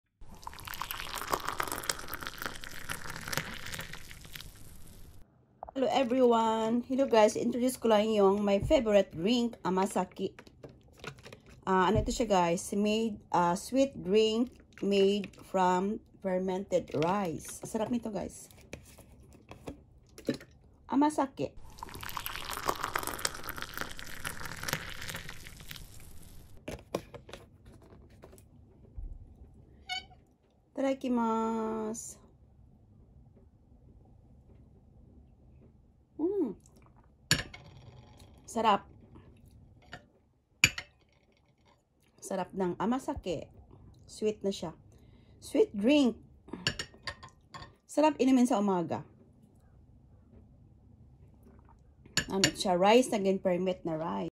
Amazake#shortvideo @ Nhins Kitchenette
Amazake a traditional sweet  low alcohol Japanese drink made from fermented rice